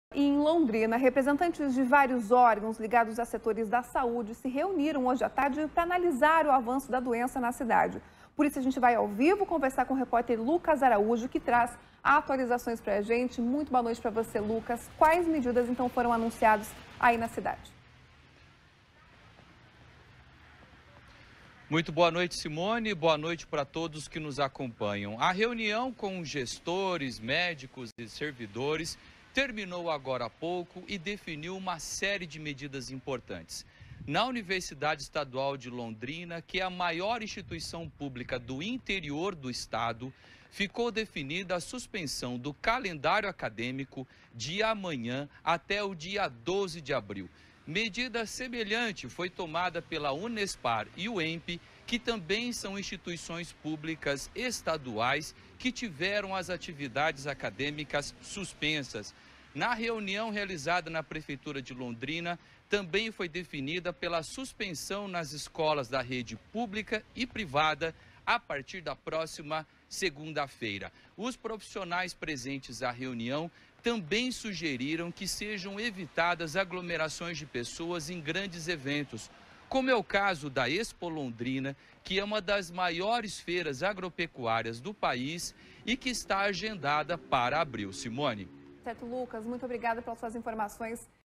Prefeitura quer adiar a ExpoLondrina: novas medidas anunciadas na cidade
Em Londrina, representantes de vários órgãos ligados ao setor da saúde se reuniram na tarde desta segunda-feira (17), para analisar o avanço da doença na cidade. Confira as medidas que foram tomadas!